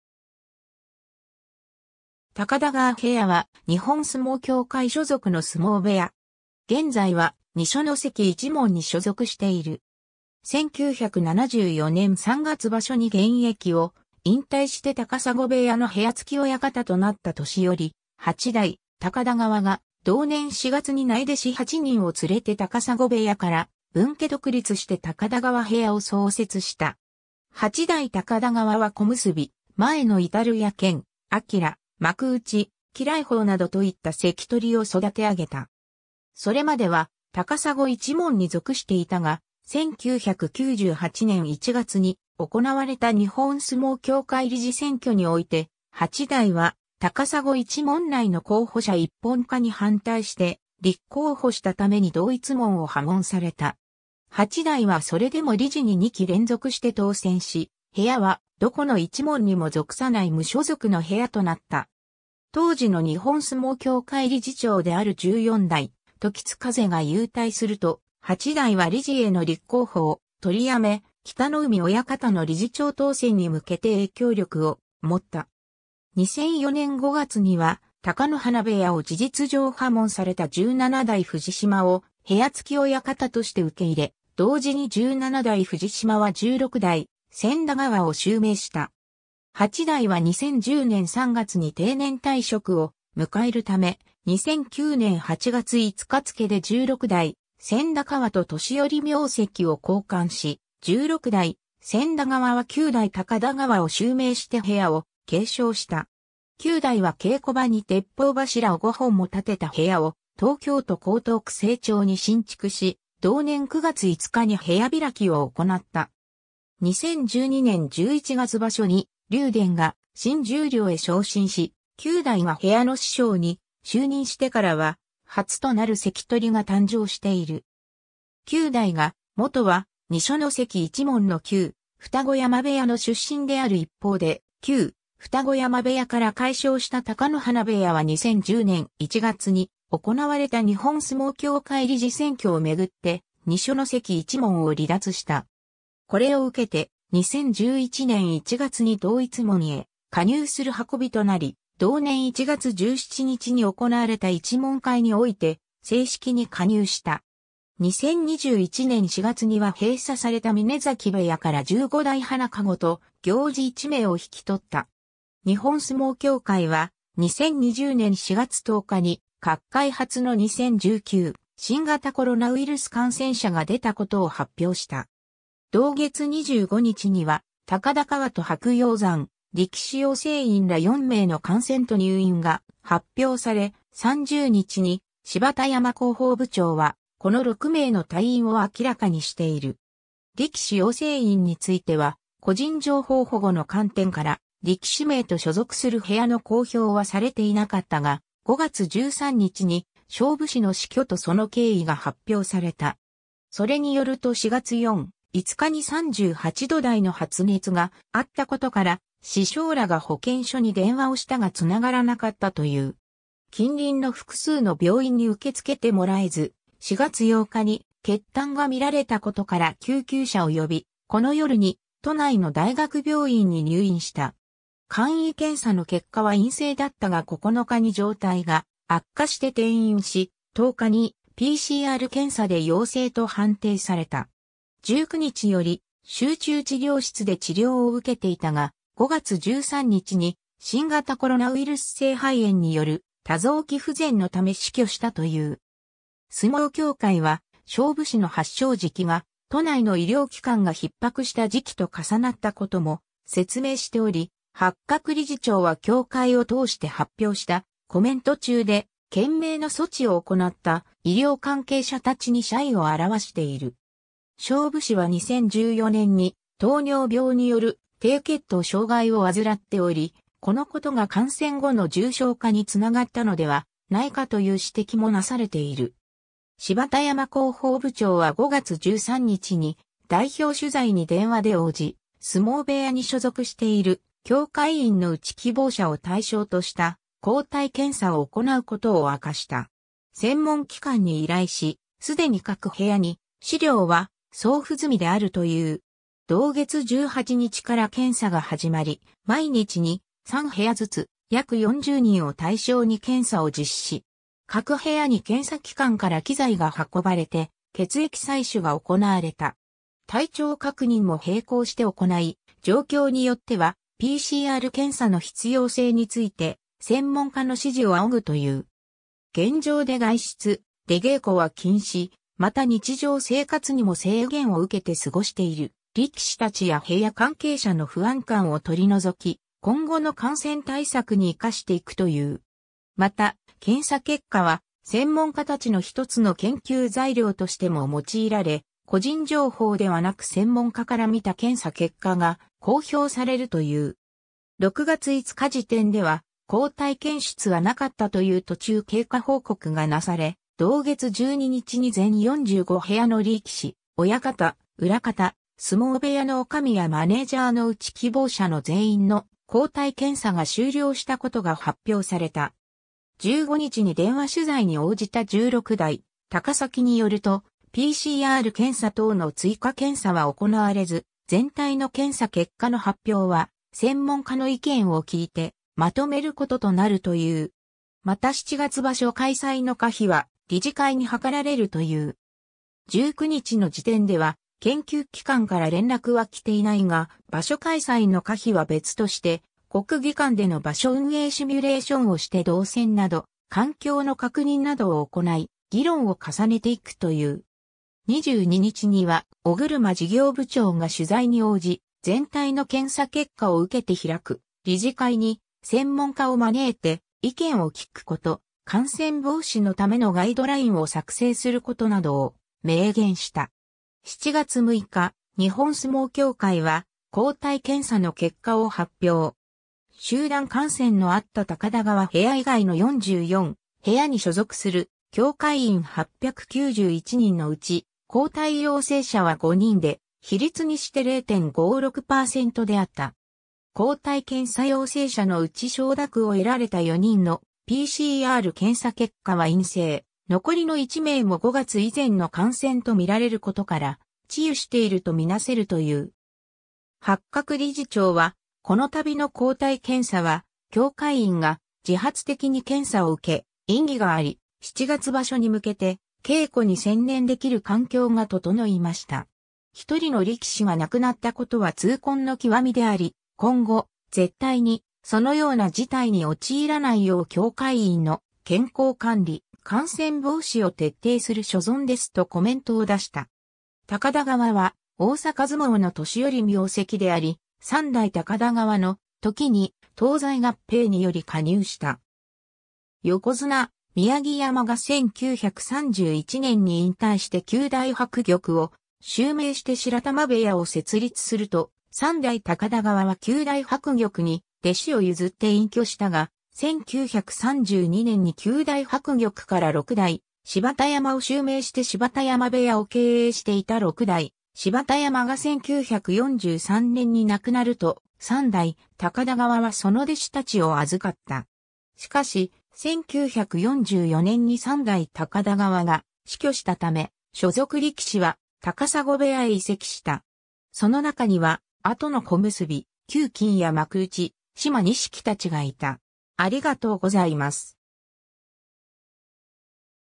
高田川部屋
高田川部屋（たかだがわべや）は、日本相撲協会所属の相撲部屋。
現在は二所ノ関一門に所属している。
 1974年（昭和49年）3月場所に現役を引退して高砂部屋の部屋付き親方となった年寄・8代高田川（元大関・前の山）が、同年4月に内弟子8人を連れて高砂部屋から分家独立して高田川部屋を創設した。
8代高田川は小結・前乃臻や剣晃、幕内・鬼雷砲などといった関取を育て上げた。
 それまでは高砂一門に属していたが、1998年（平成10年）1月に行われた日本相撲協会理事選挙において、8代は高砂一門内の候補者一本化に反対して立候補したために同一門を破門された。
8代はそれでも理事に2期連続して当選し、部屋はどこの一門にも属さない無所属の部屋となった。
当時の日本相撲協会理事長である14代時津風（元大関・豊山）が勇退すると、8代は理事への立候補を取り止め、北の湖親方（元横綱・北の湖）の理事長当選に向けて影響力を持った。
2004年5月には貴乃花部屋を事実上破門された17代藤島（元関脇・安芸乃島）を部屋付き親方として受け入れ、同時に17代藤島は16代千田川を襲名した。
 8代は2010年3月に定年退職を迎えるため、2009年8月5日付で16代千田川と年寄名跡を交換し、16代千田川は9代高田川を襲名して部屋を継承した。
9代は稽古場に鉄砲柱を5本も立てた部屋を東京都江東区清澄に新築し、同年9月5日に部屋開きを行った。
2012年11月場所に竜電が新十両へ昇進し、9代が部屋の師匠に就任してからは初となる関取が誕生している。
 9代が元は二所ノ関一門の旧・二子山部屋の出身である一方で、旧・二子山部屋から改称した貴乃花部屋は2010年1月に行われた日本相撲協会理事選挙を巡って二所ノ関一門を離脱した。
これを受けて、2011年1月に同一門へ加入する運びとなり、同年1月17日に行われた一門会において正式に加入した。
 2021年4月には閉鎖された峰崎部屋から15代花籠と行司1名を引き取った。
 日本相撲協会は、2020年4月10日に角界初の2019新型コロナウイルス感染者が出たことを発表した。
同月25日には、高田川と白鷹山、力士養成員ら4名の感染と入院が発表され、30日に芝田山広報部長はこの6名の退院を明らかにしている。
 力士養成員については個人情報保護の観点から力士名と所属する部屋の公表はされていなかったが、5月13日に勝武士の死去とその経緯が発表された。
 それによると4月4、5日に38度台の発熱があったことから、師匠らが保健所に電話をしたがつながらなかったという。
近隣の複数の病院に受け付けてもらえず、4月8日に血痰が見られたことから救急車を呼び、この夜に都内の大学病院に入院した。
簡易検査の結果は陰性だったが9日に状態が悪化して転院し、10日にPCR検査で陽性と判定された。
19日より集中治療室で治療を受けていたが、5月13日に新型コロナウイルス性肺炎による多臓器不全のため死去したという。
 相撲協会は勝武士の発症時期が都内の医療機関がひっ迫した時期と重なったことも説明しており、八角理事長は協会を通して発表したコメント中で懸命の措置を行った医療関係者たちに謝意を表している。
勝武士は2014年に糖尿病による低血糖障がいを患っており、このことが感染後の重症化につながったのではないかという指摘もなされている。
 芝田山広報部長は5月13日に代表取材に電話で応じ、相撲部屋に所属している協会員(力士、親方、呼び出し、床山、行司、世話人、若者頭)のうち希望者を対象とした抗体検査を行うことを明かした。
専門機関に依頼し、既に各部屋に資料は送付済みであるという。
同月18日から検査が始まり、毎日2、3部屋ずつ約40人を対象に検査を実施。
各部屋に検査機関から機材が運ばれて血液採取が行われた。
体調確認も並行して行い、状況によってはPCR検査の必要性について専門家の指示を仰ぐという。
現状で外出・出稽古は禁止、また日常生活にも制限を受けて過ごしている力士たちや部屋関係者の不安感を取り除き、今後の感染対策に生かしていくという。
また検査結果は専門家たちの1つの研究材料としても用いられ、個人情報ではなく専門家から見た検査結果が公表されるという。
 6月5日時点では、抗体検出は無かったという途中経過報告がなされ、同月12日に全45部屋の力士・親方・裏方、相撲部屋のおかみやマネージャーのうち希望者の全員の抗体検査が終了したことが発表された。
15日に電話取材に応じた16代高崎によると、PCR...